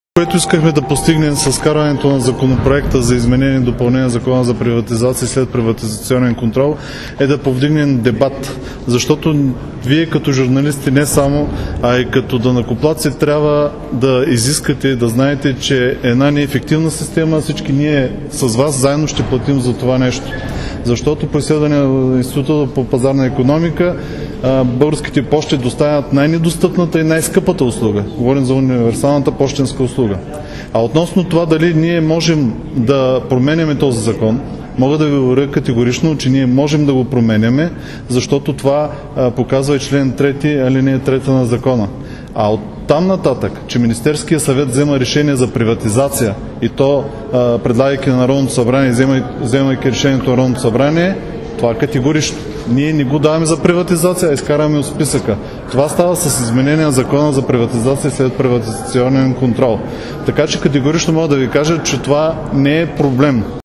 Николов- Български пощи - НС - коментар
Зам.-председателят на ПГ на ГЕРБ Валентин Николов коментира оттеглянето от дневния ред на НС на ЗИД на Закона за приватизация и следприватизационен контрол, с който трябваше 4 предприятия да бъдат извадени от забранителния списък за приватизация.